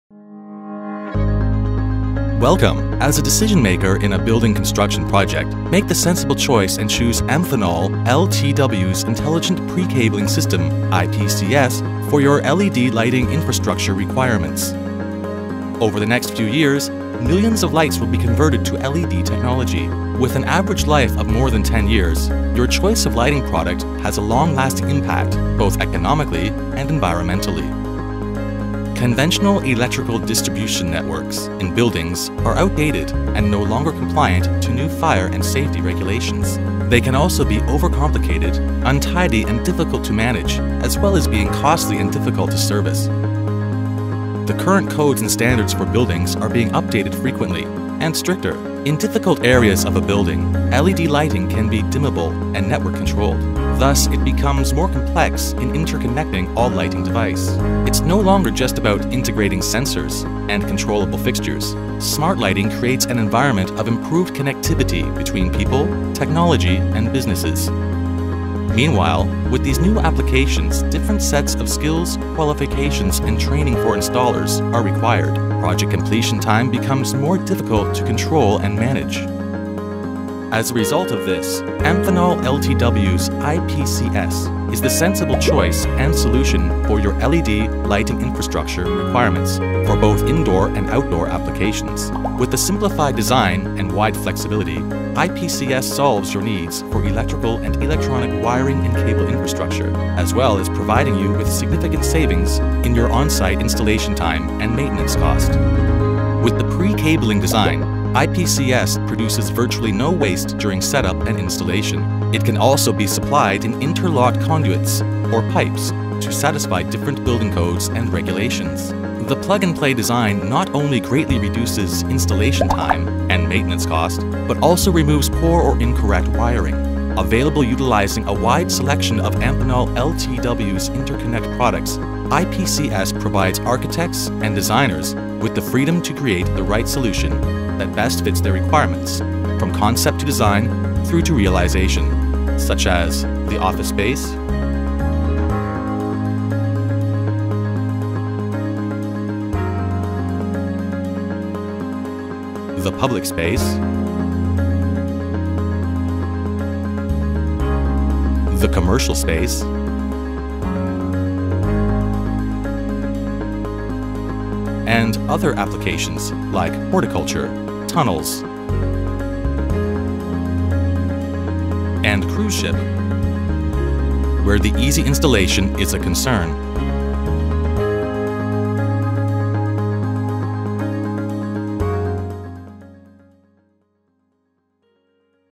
Amphenol LTW Intelligent Pre-Cabling System (IPCS)
Amphenol LTW’s “Intelligent Pre-Cabling System” (IPCS) is the sensible choice and solution for your cabling infrastructure requirements; for both indoor and outdoor applications with IP68 waterproof rating. Available utilizing a wide selection of Amphenol LTW’s interconnect products, IPCS provides key advantages such as long life and minimal maintenance. It serves designers with peace of mind, allowing them the freedom to create and innovate interconnection wonders.